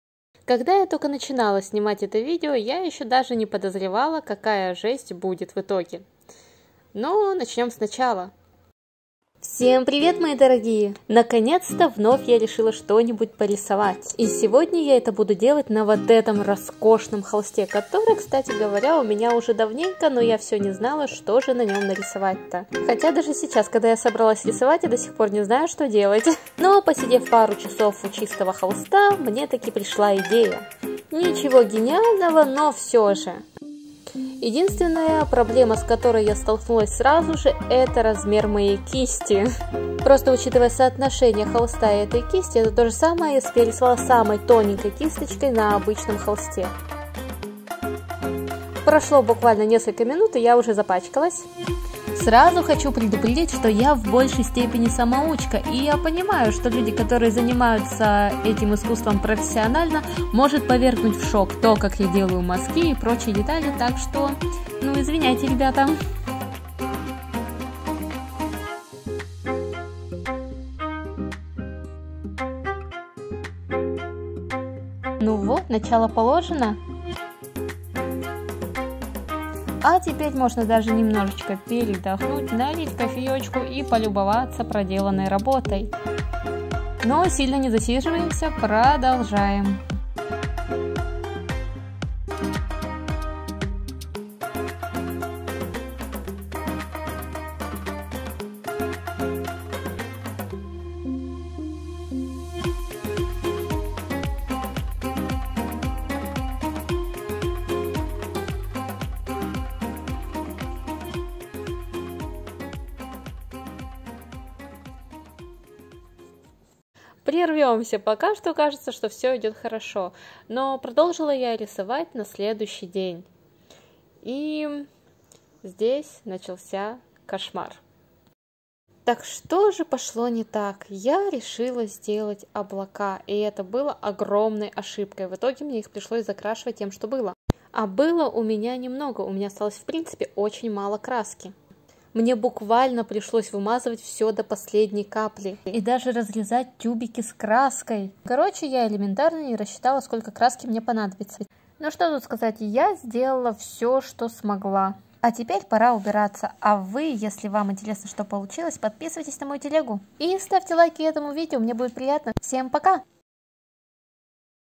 Художественный коллапс🤯(Опять всё пошло не по плану😅)
Решила в коей-то веки порисовать, но неудачно 😅, а всё так хорошо начиналось)